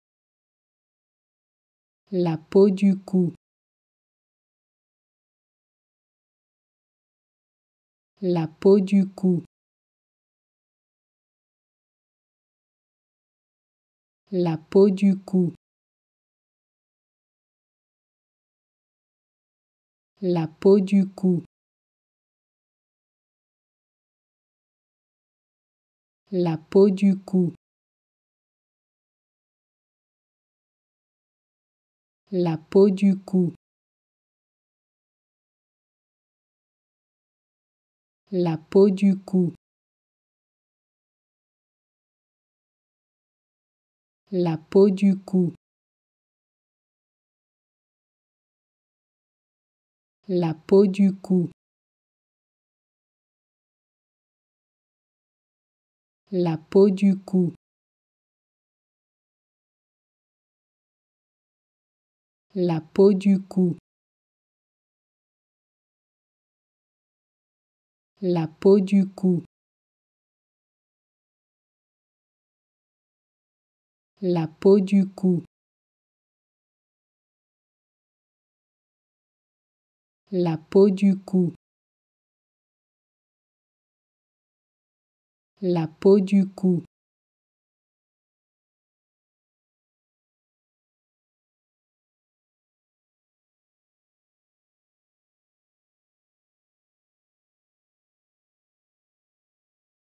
How to Pronounce ''La peau du cou'' (The skin on the neck) Correctly in French
Learn how to say and properly pronounce ''La peau du cou'' in French with this free pronunciation tutorial.